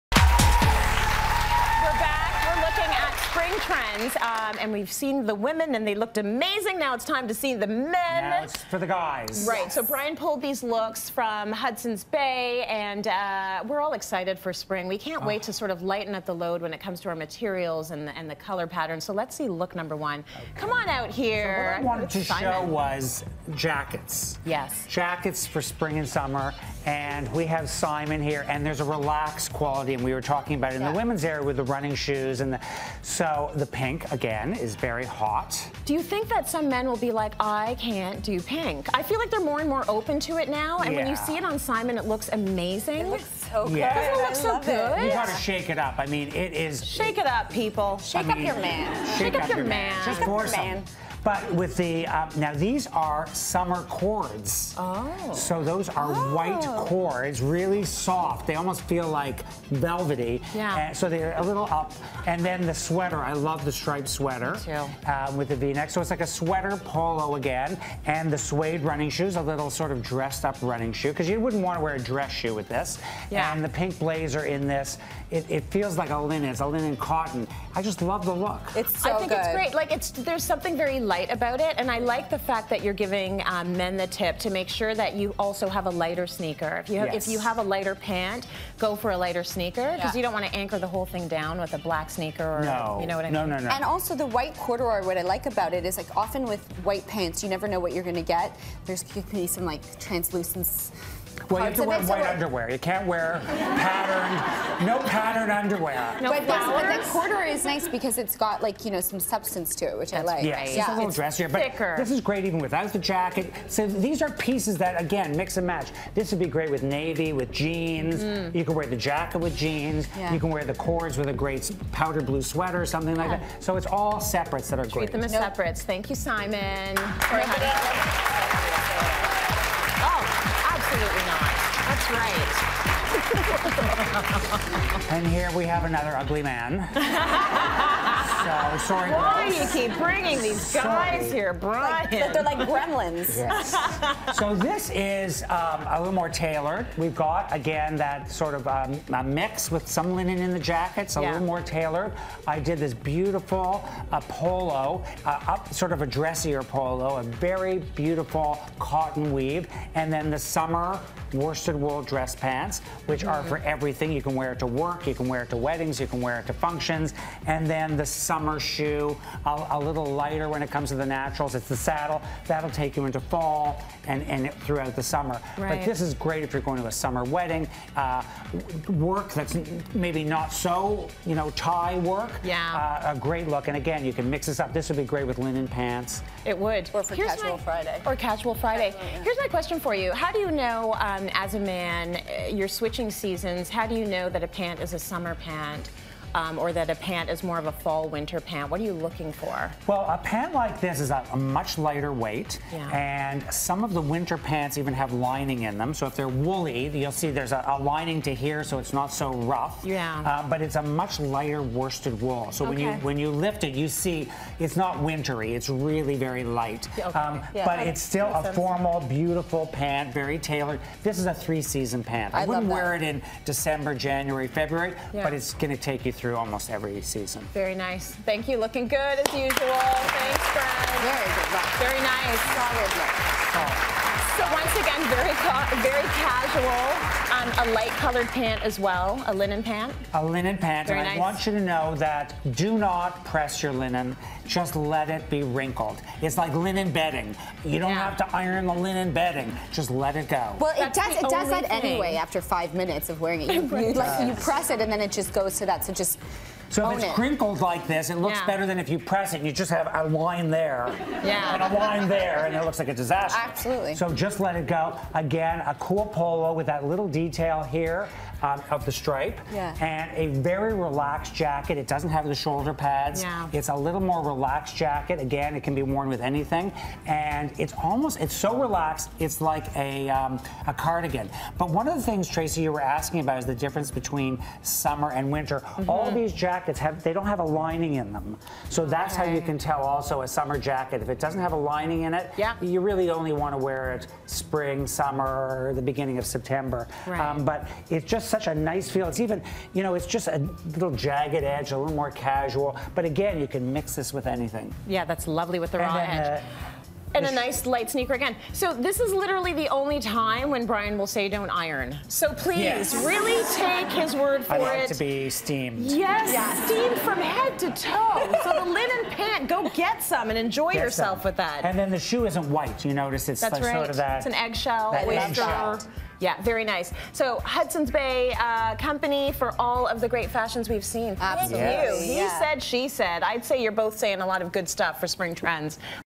6 menswear spring outfits that will take you from work to happy hour
Designer, Brian Guckstein, has 6 ways to wear spring trends anywhere this spring from work to patio happy hour.